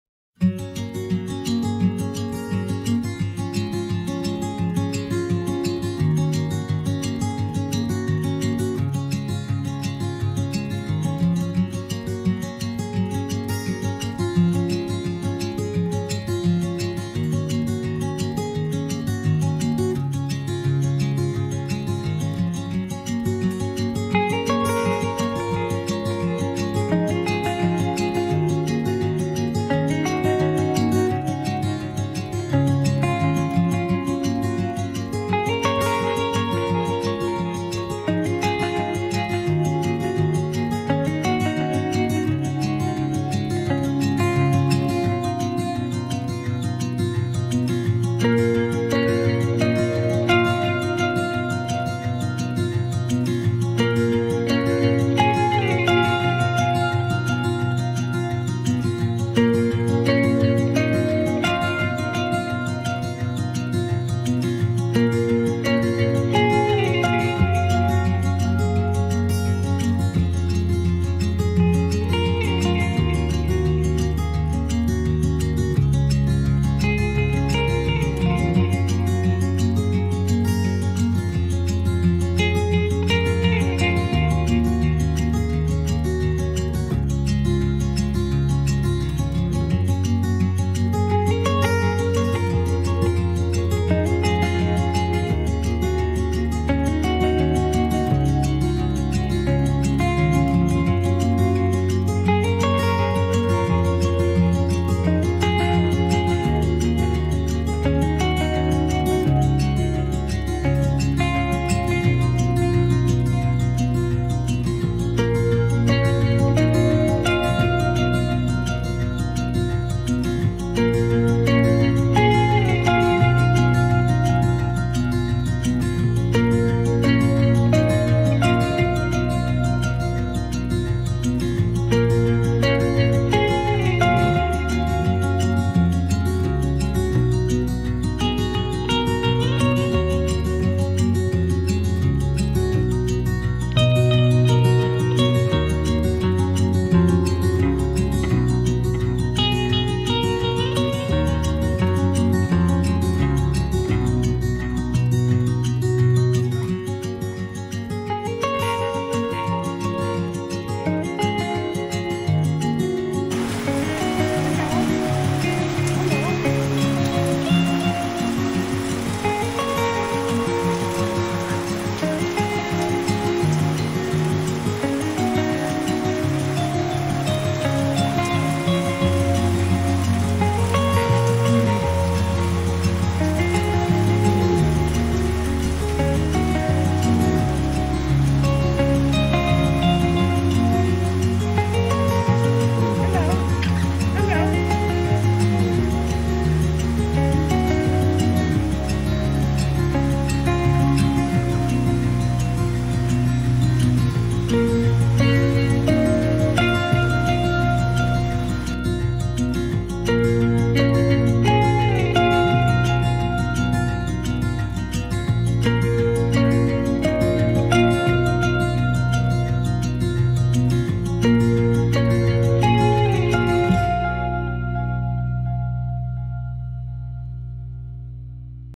Bohol's Iconic Attractions: Sikatuna - Mirror Of The World Bohol
Bohol in the Philippines visiting the  "Mirror of the World"  - come along and enjoy with us.